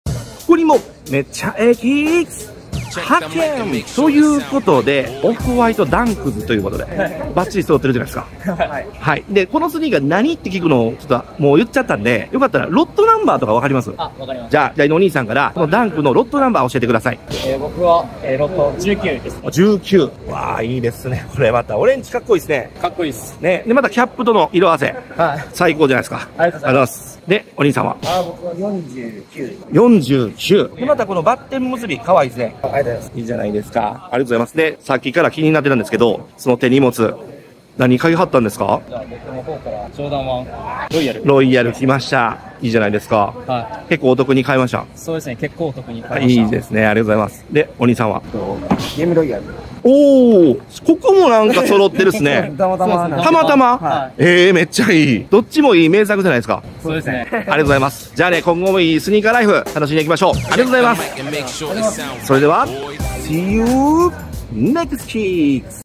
スニーカー街ロケ Meccha A Kicks No.383 ＠Osaka
街でMeccha A Kicksを履いてる人にインタビュー！

スニーカーの魅力を最大限伝える写真を投稿！

普段Tiktokをよく見る人はコチラをどうぞ！

《企画運営》
株式会社ジー